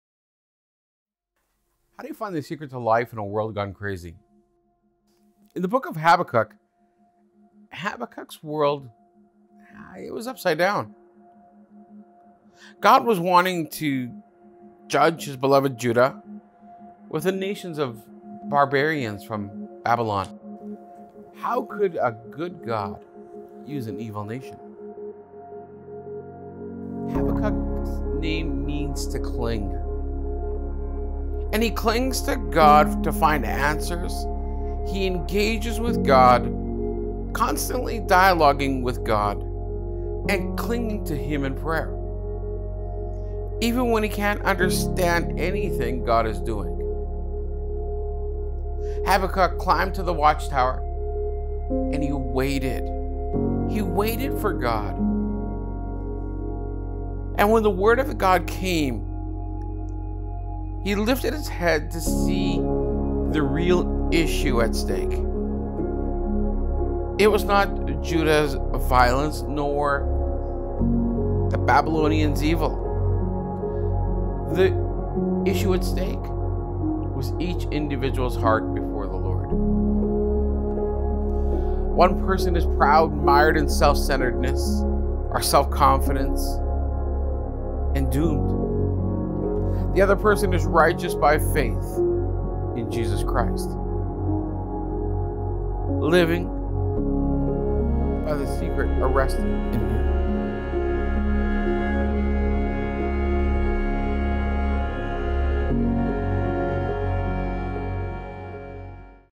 Habakkuk Series Trailer
May 26, June 2, & June 23 2019 we are going into Habakkuk.  Join us at 10:00AM